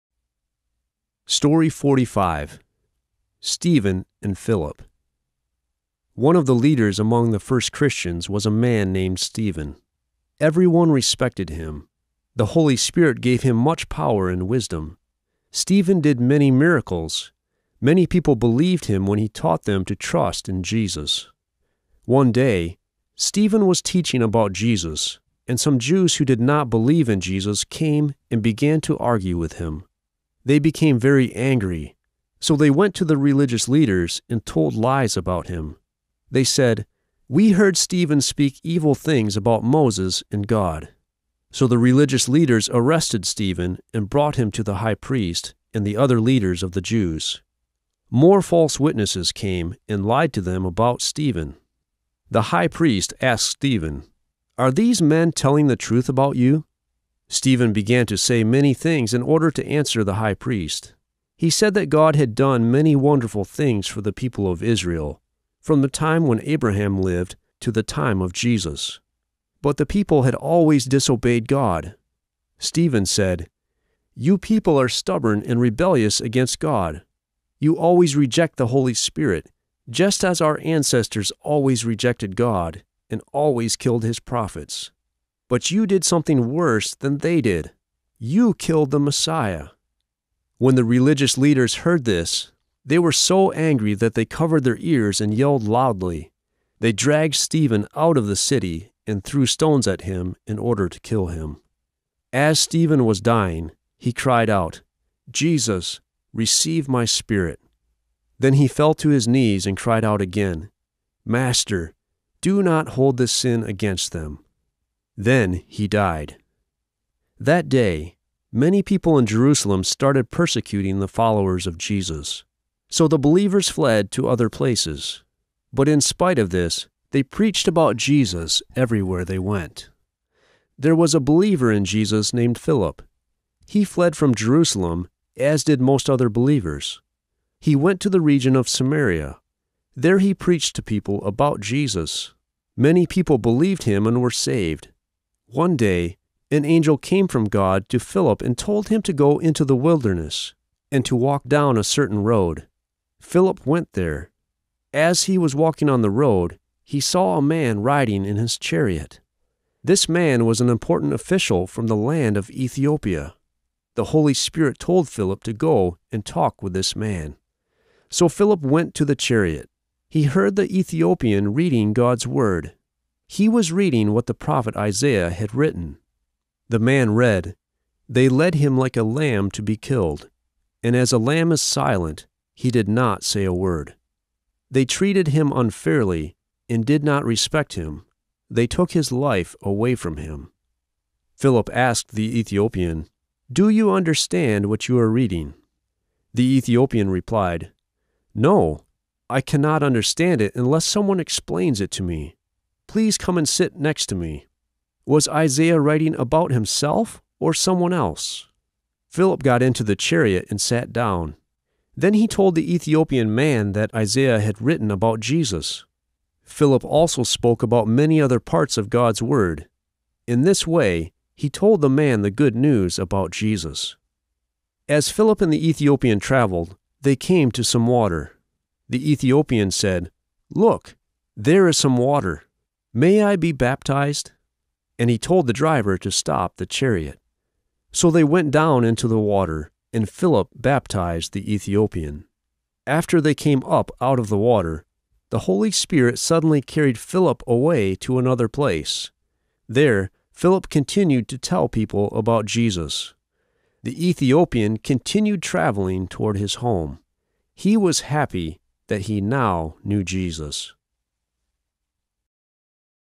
Stephen and Phillip| Animated Bible Story| Makayi
Animated Story Stephen and Phillip Unfolding Word open bible stories

(Attribution-Sweet-Publishing/Free bible images)